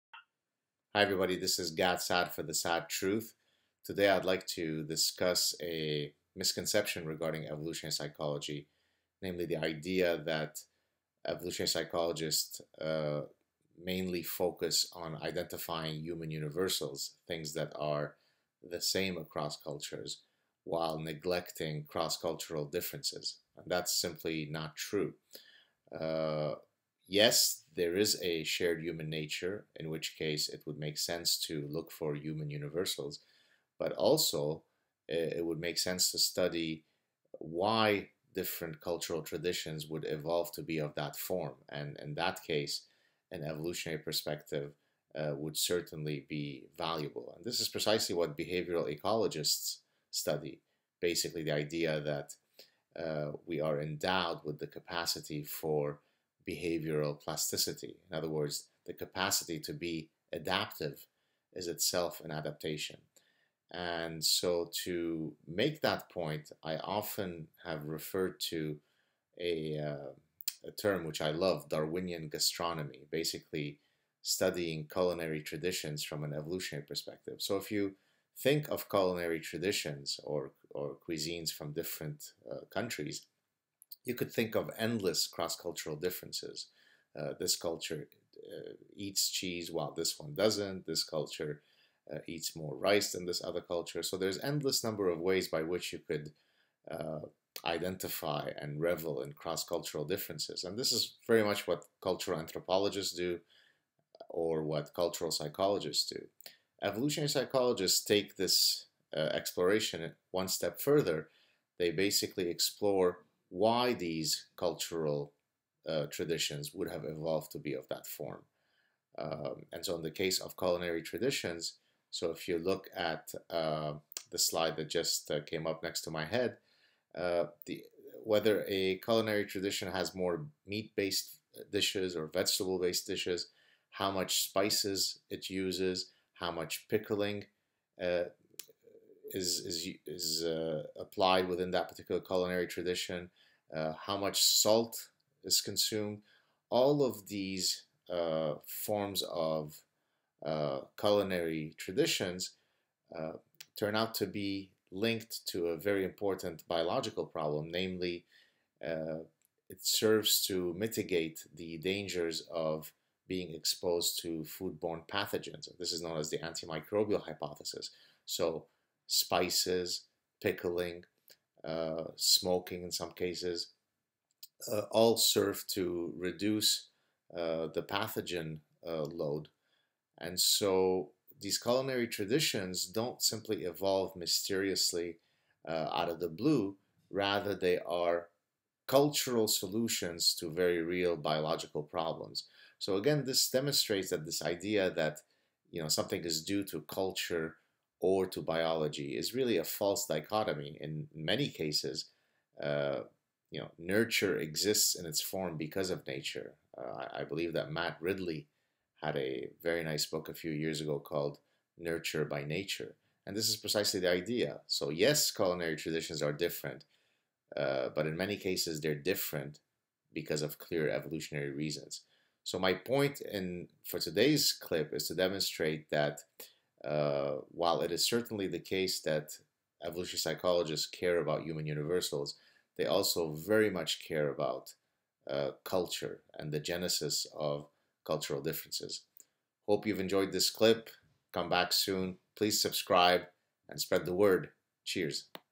THE SAAD TRUTH_17: Food Preparation and Human Nature
Is there a biological explanation for the wide range of culinary traditions found around the world (e.g., the extent of spice use)? Yes, it's all about the management of foodborne pathogens across different local niches.